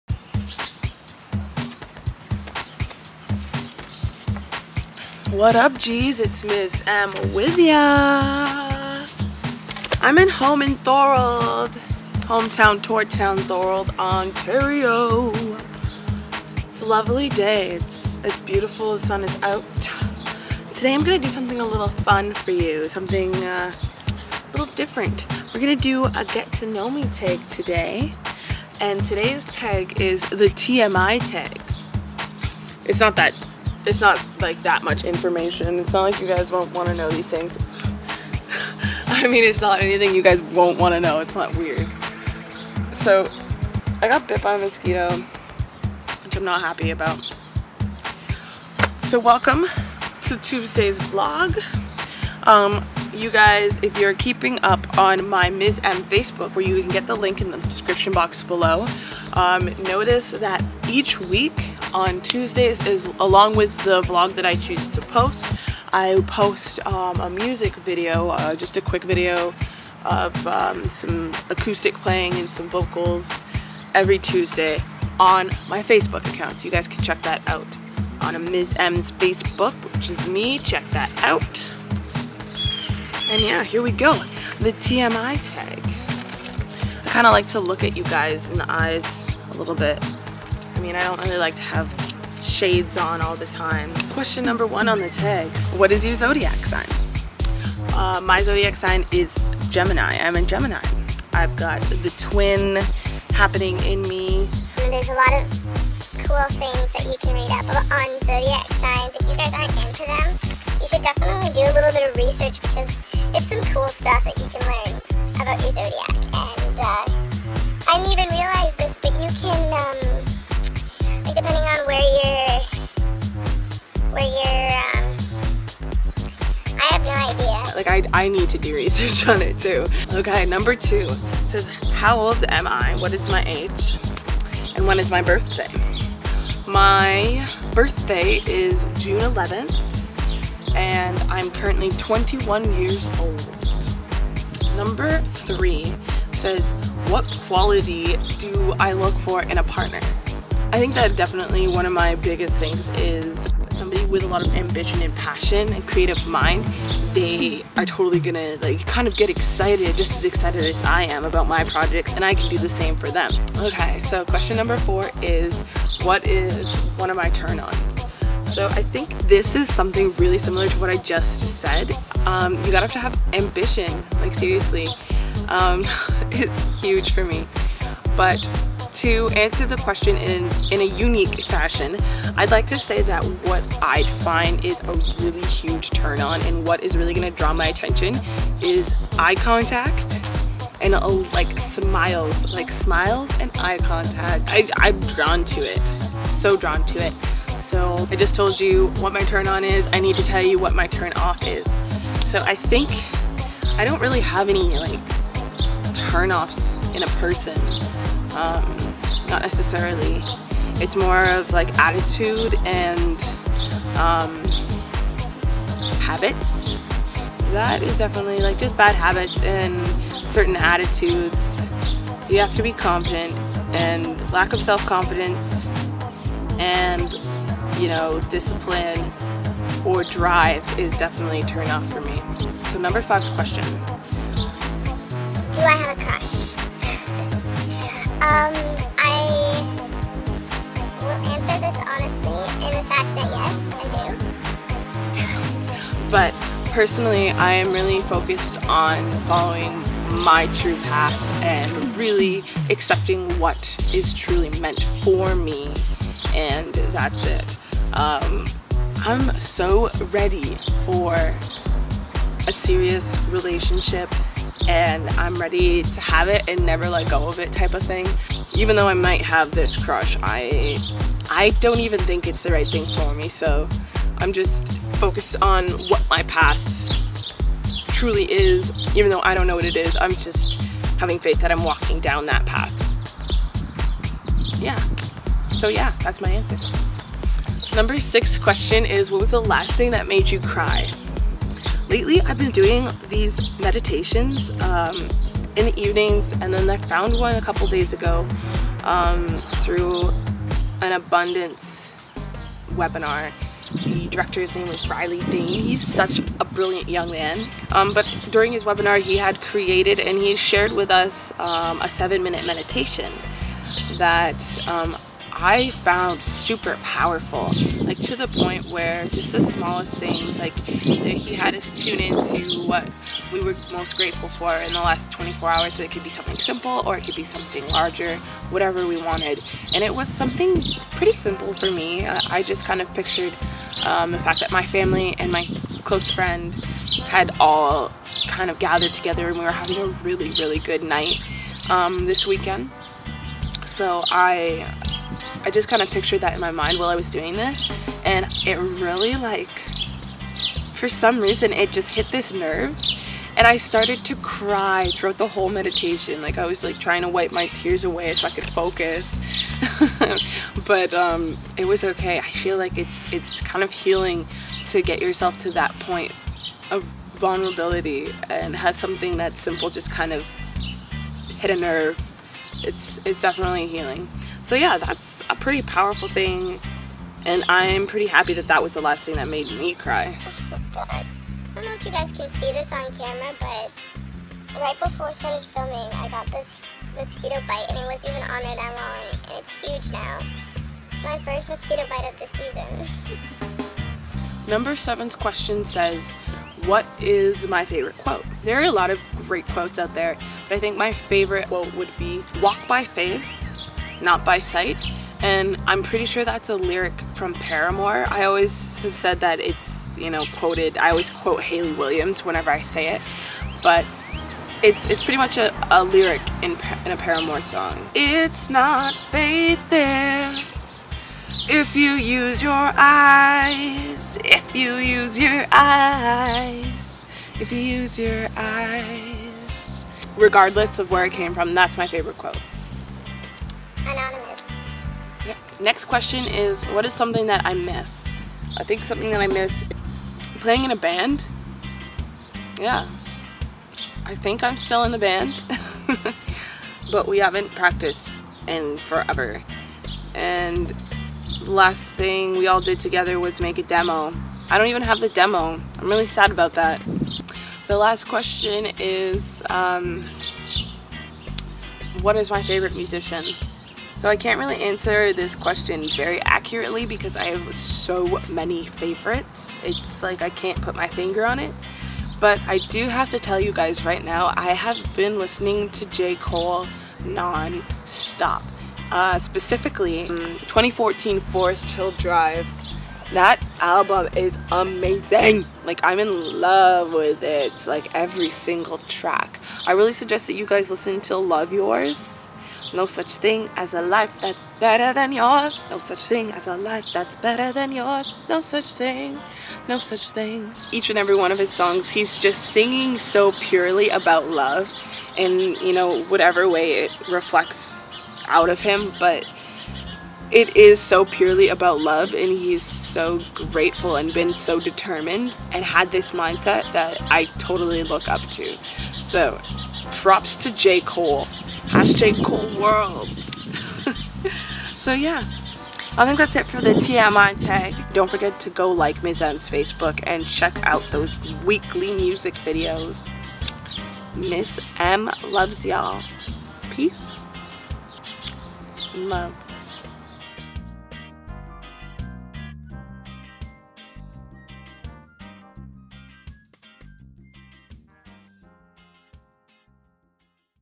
TMI Tag | Get To Know Me Series
This is the TMI Tag! Don't worry, it's not that type of weird stuff you don't want to hear! I answer 10 basic questions! Get To Know Me! - LINKS BELOW ~

✮More Vlogs✮